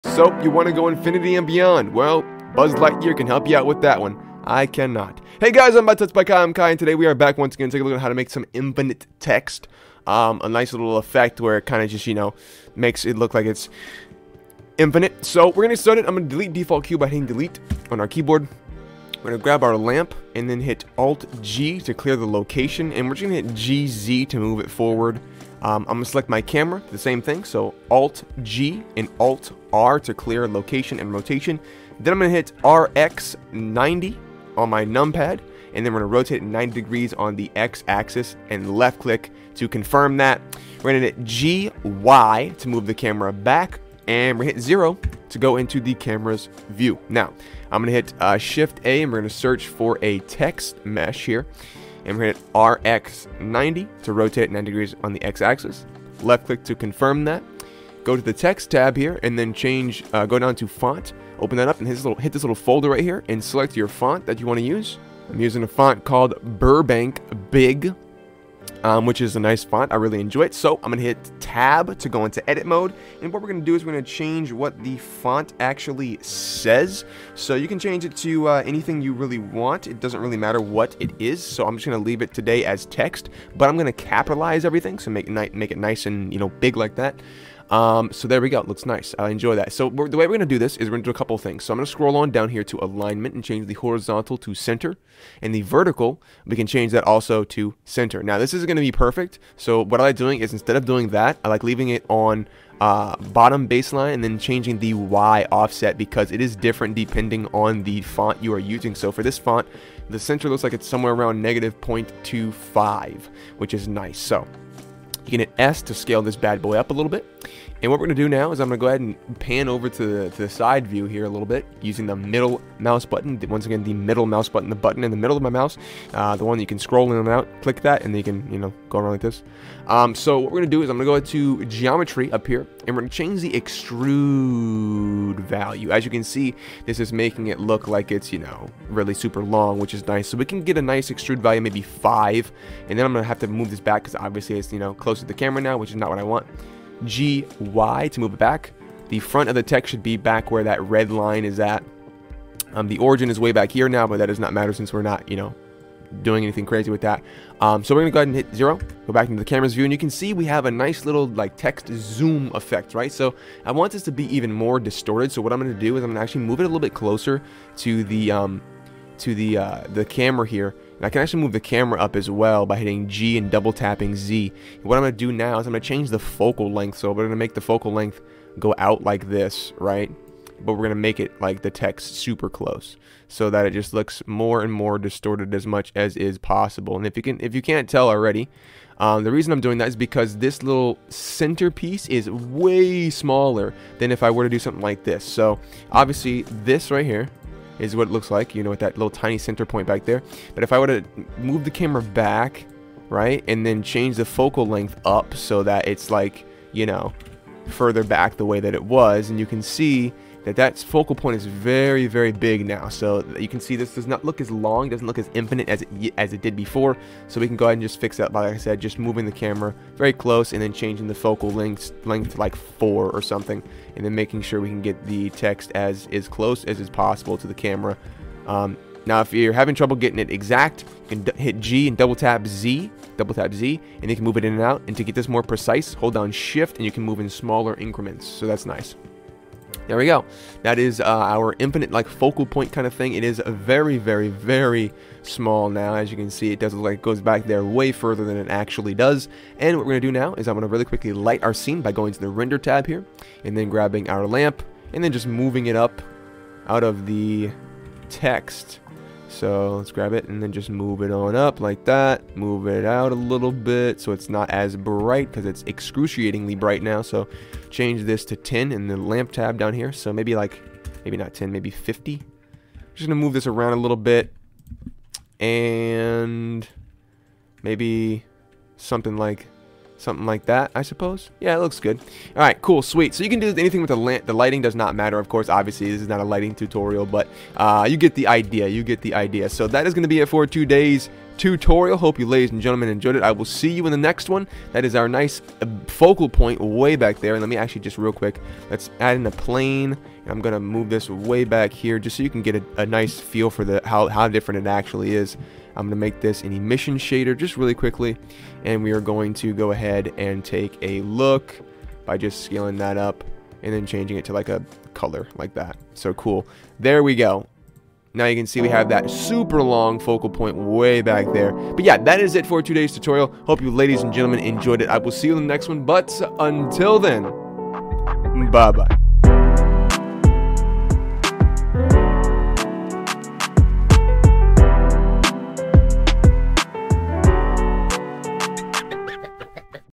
Infinite Long Text Effect in Blender 2.9 Eevee - Tutorial
Here's a tutorial on how to make an infinite long text effect in Blender 2.9 Eevee!

My name's Kai and I create short, simple tutorials to help you create your own art.